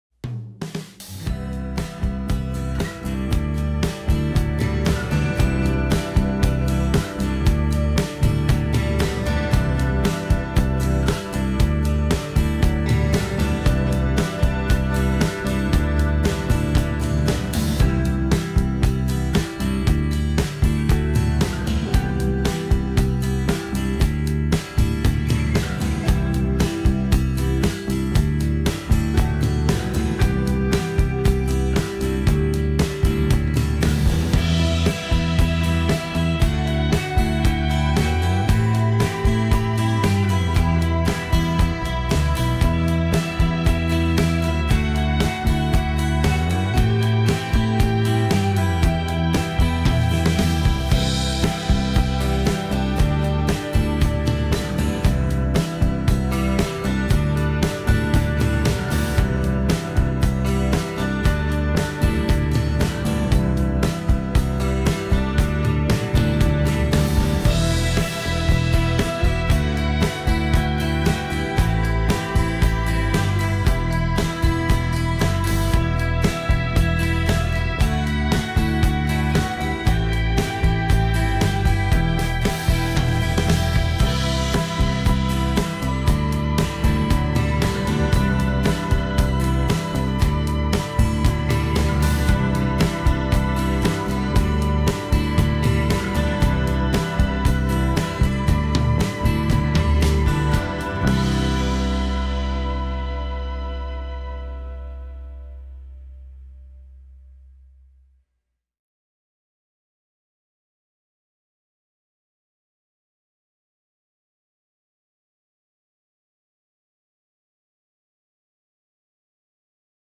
Glentrool and Loch Trool in rural Dumfries and Galloway, Scotland, DJI Mavic 2 Pro 4K video footage
4K drone footage of the rural area around Glentrool and Loch Trool in Dumfries and Galloway, south west Scotland.
Video captured with a DJI Mavic 2 Pro fitted with Polar Pro ND filters.